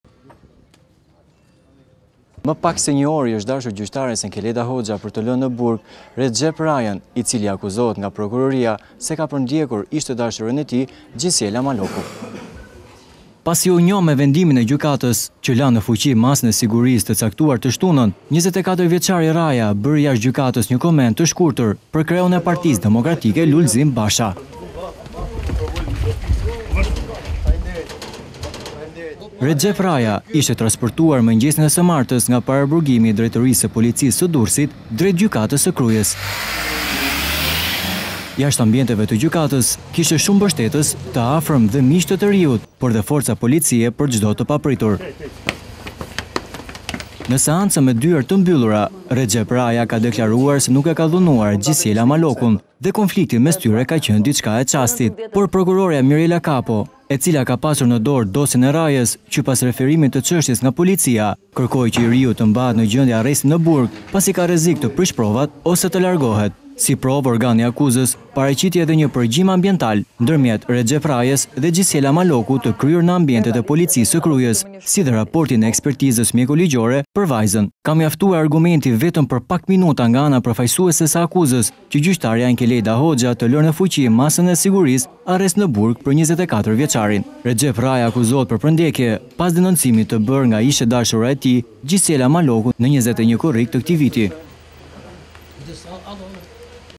Rexhep Rraja mbetet në qeli, Gjykata e Krujes konfirmon arrestin me burg| ABC News Albania
Pasi u njoh me vendimin e gjykatës që la në fuqi masën e sigurisë të caktuar të shtunën, 24-vjeçari  Rraja bëri jashtë gjykatës një koment të shkurtër për kreun e Partisë Demokratike, Lulzim Basha.

24-vjeçari Rrexhep Rraja ishte transportuar mëngjesin e së martës nga paraburgimi i drejtorisë së policisë së Durrësit drejt Gjykates së Krujës.

Jashtë ambienteve të gjykatës, kishte shumë mbështetës, të afërm dhe miq të të riut, por edhe forca policie për çdo të papritur.

Në  seancën me dyer të mbyllura Rexhep Rraja ka deklaruar se nuk e ka dhunuar Xhisiela Malokun dhe konflikti mes tyre ka qenë diçka e momentit.

Por prokurorja Mirela Kapo, e cila ka pasur ne dore dosjen e Rrajes qe pas referimit te  ceshtjes nga policia, kërkoi që i riu te mbahet në gjendje “arresti ne burg”, pasi ka rrezik të prishë provat ose mund të largohet. Si provë, organi i akuzës paraqiti edhe një përgjim ambiental ndërmjet Rrexhep Rrajas dhe Xhisiela Malokut të kryer në ambientet e prokurorisë së Krujës, si edhe raportin e ekspertizës mjeko-ligjore kryer për vajzën.

Ka mjaftuar argumentimi vetëm për pak minuta nga ana e përfaqësueses së akuzës që edhe gjyqtarja Enkelejda Hoxha të lërë në fuqi masën e sigurisë “arrest në burg” për 24 vjeçarin.

Rrexhep Rraja akuzohet për “përndjekje”, pas denoncimit të bërë nga ish- e dashura e tij Xhisiela Maloku, në 21 korrik të këtij viti.

Gazetar ABC News: Andi Qosja

/abcnews.al